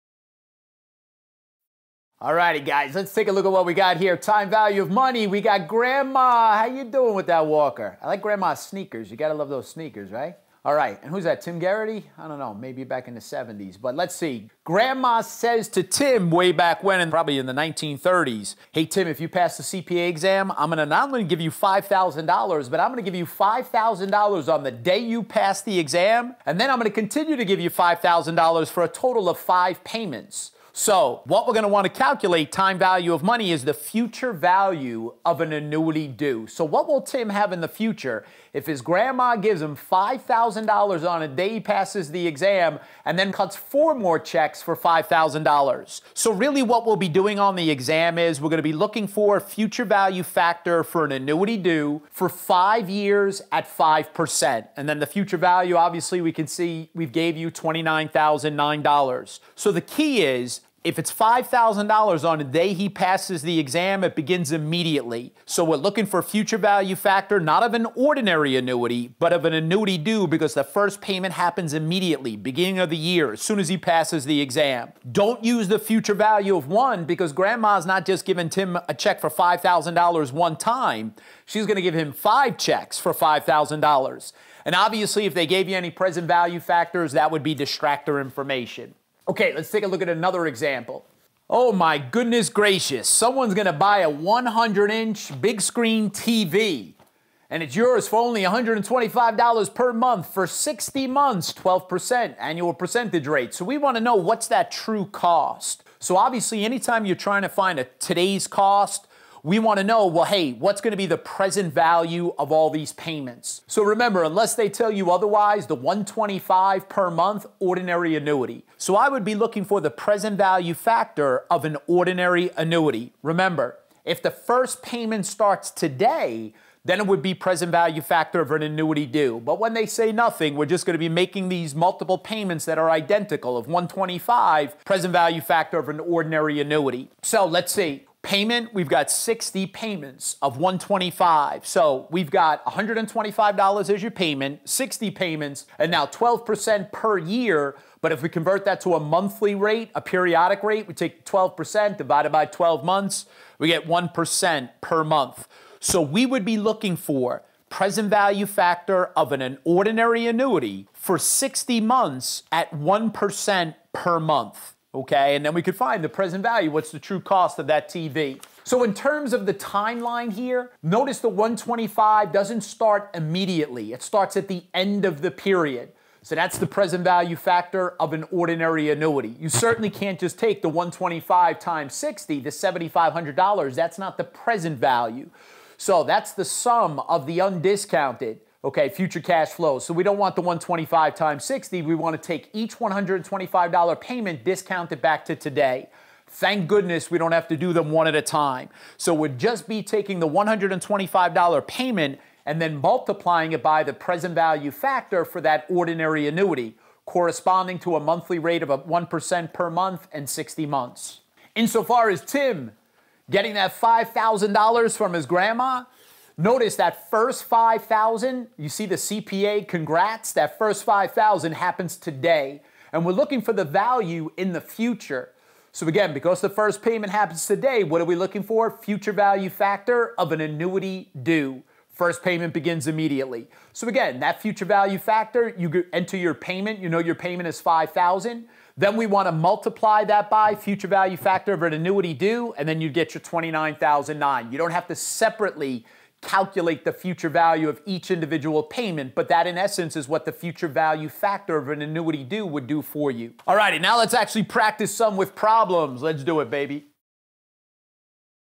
FAR: Balance Sheet, EPS and Cash Flows: The Time Value of Money
Studying for the CPA Exam? Watch Becker’s Skills Practice video to learn about FAR: Balance Sheet, EPS and Cash Flows: The Time Value of Money.

These videos were part of a previous version of Becker’s learning curriculum and may reference materials that no longer exist.